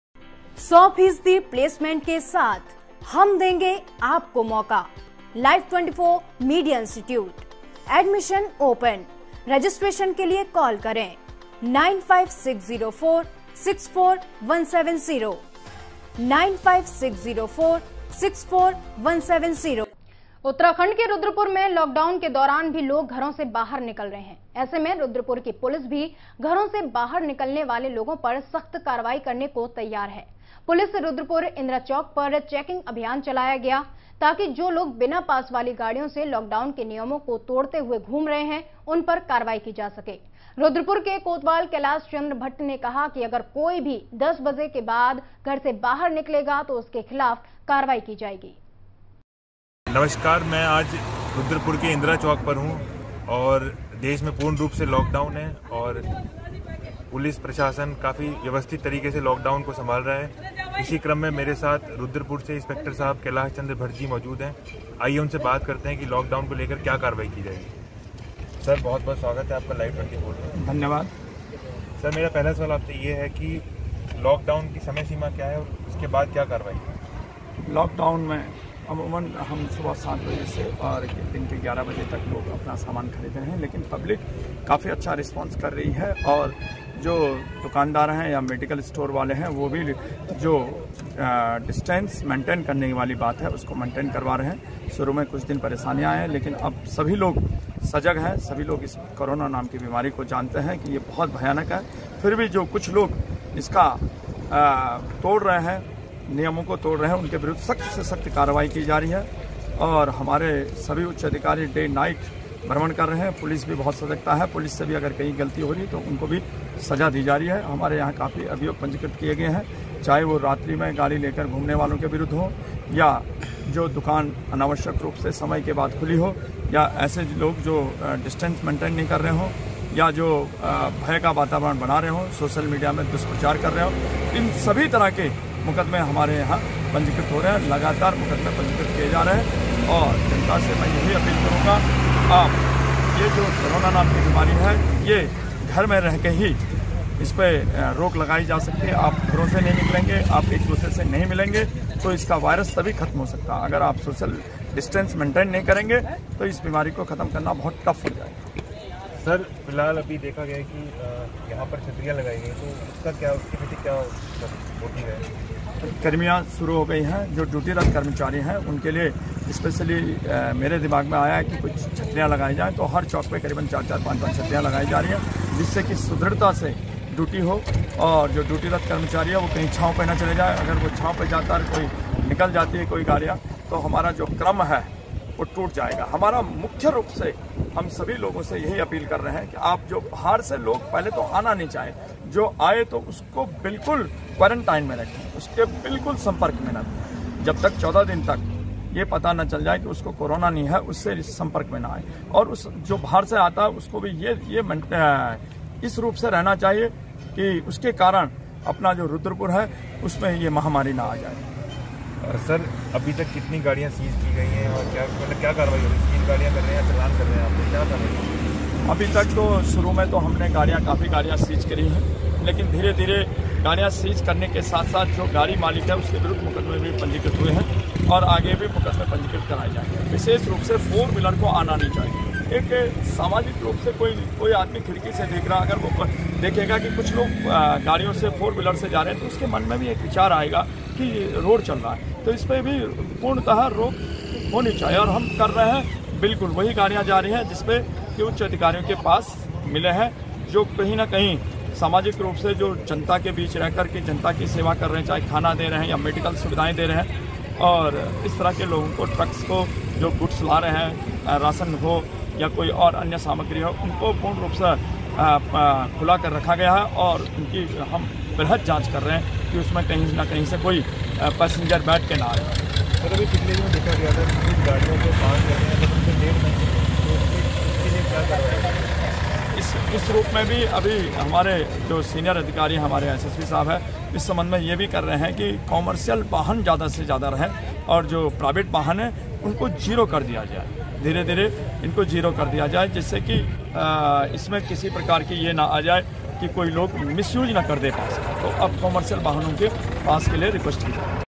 लॉकडाउन पर रुद्रपुर पुलिस सख्त
उत्तराखंड के रुद्रपुर में लॉकडाउन के दौरान भी लोग घरों से बाहर निकल रहे है...ऐसे में रुद्रपुर की पुलिस भी घरों से बाहर निकलने पर सख्त कार्रवाई करने को तैयार है...पुलिस रुद्रपुर  इंद्रा चौक पर चेकिंग अभियान हुआ और शुरू किया है ताकि जो बिना पास वाली गाड़ियों से लॉकडाउन के नियमों को तोड़ते हुए घूम रहे है उन पर कार्रवाई की जा सके...रूद्रपुर के कोतवाल कैलाश चंद्र भट्ठ ने कहा कि अगर कोई भी 10 बजे के बाद घर से बाहर  निकलेगा उसके खिलाफ कार्यवाई की जाएगी...